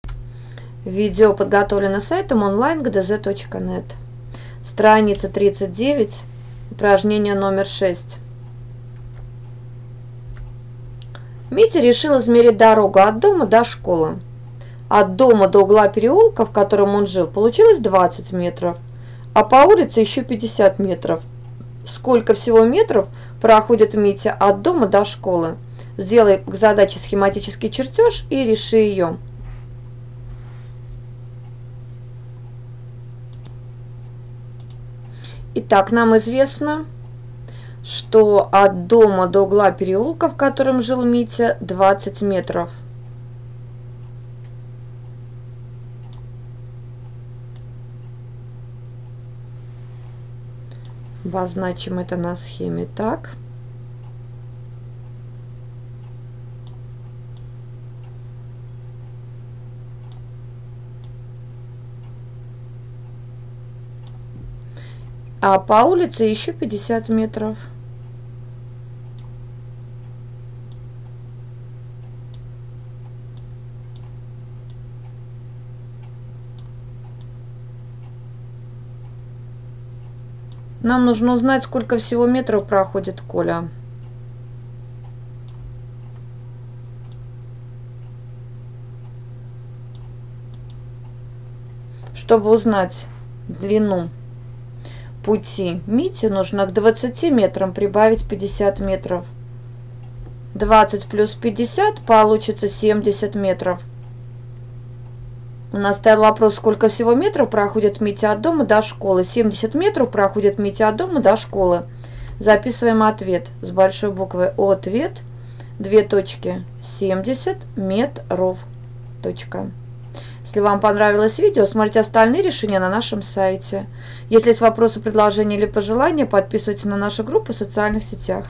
Страница 39 Задание 6 – Математика 2 класс (Моро) Часть 1
Если хотите выполнить упражнение №6 на странице 39, вы должны уметь решать простые задачи.